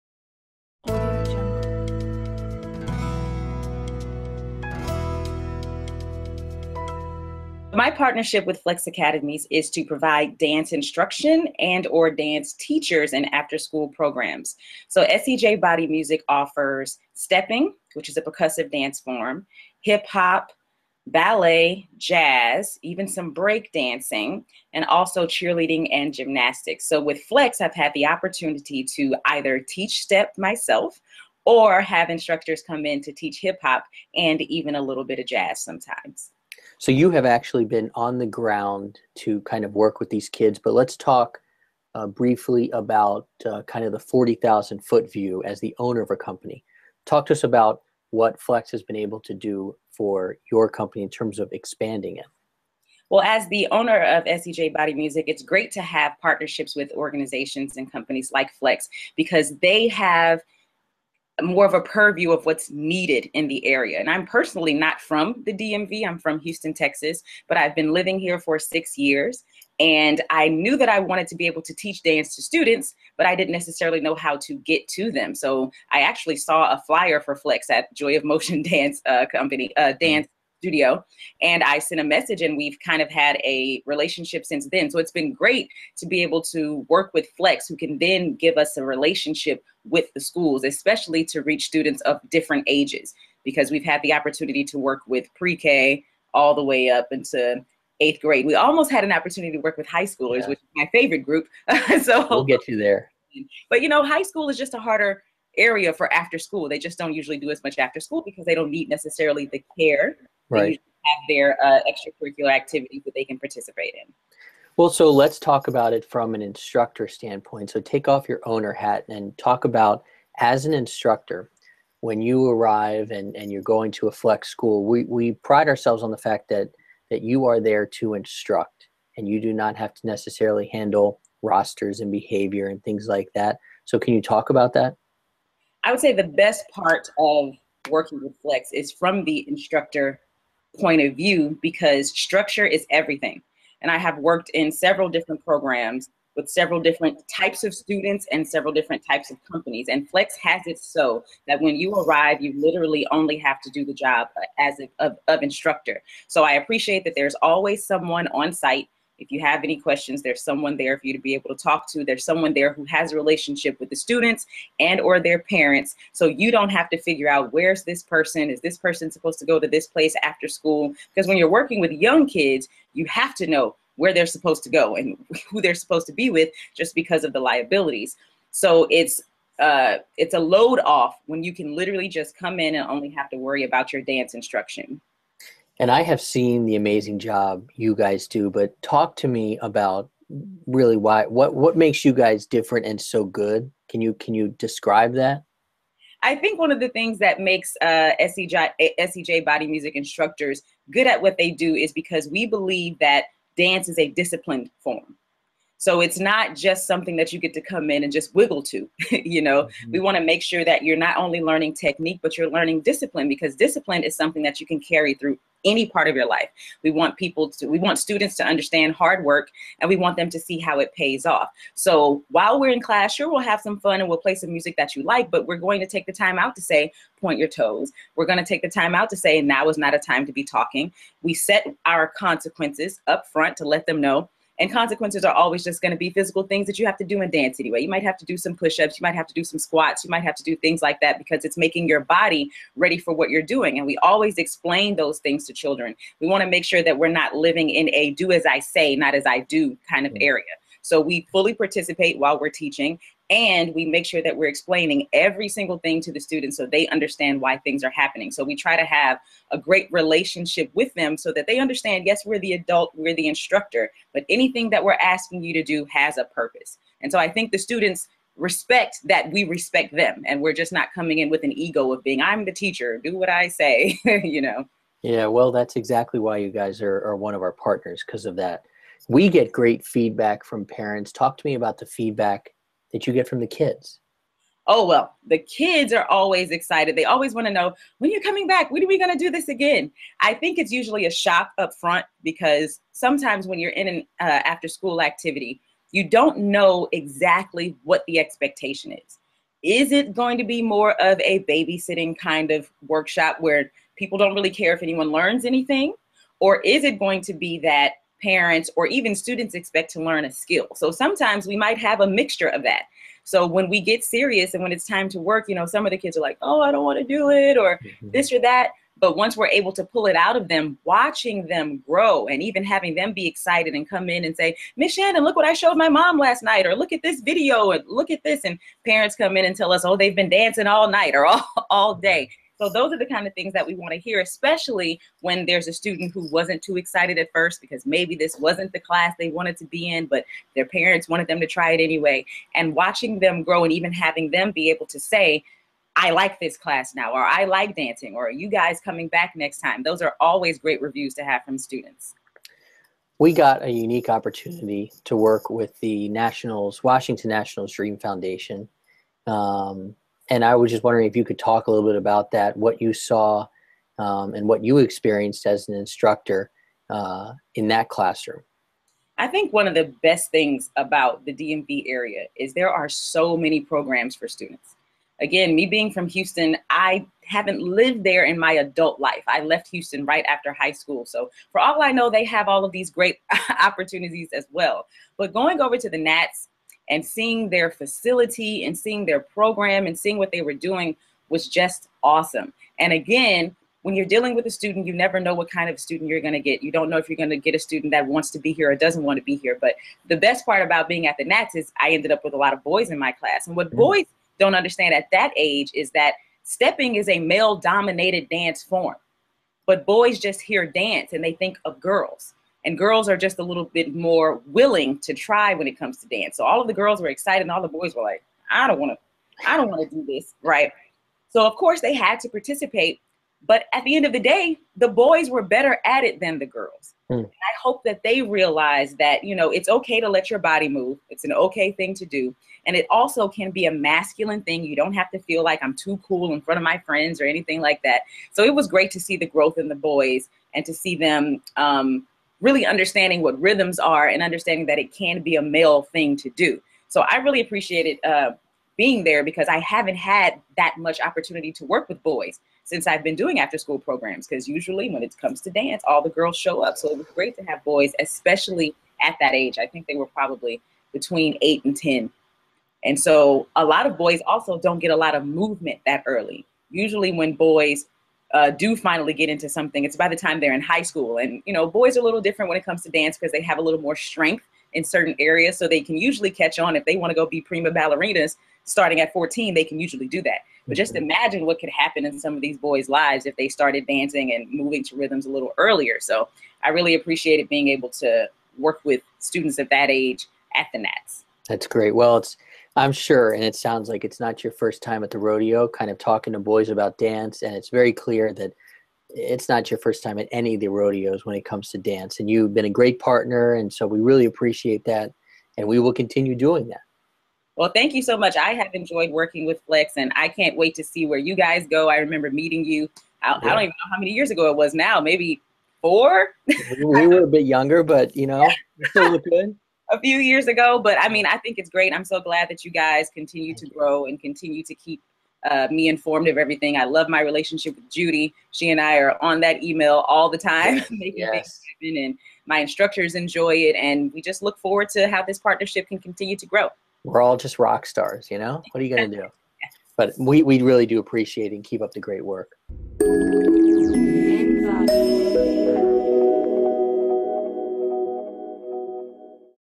Vendor Shannan Johnson describes the benefits of her partnership with Flex
Shannan Johnson, Founder of SEJ Body Music, says that her company's partnership with Flex has allowed her to pursue her passion, teaching children how to dance.  Johnson says that Flex offers an afterschool program "structure" that relieves her and her staff of paperwork and management details.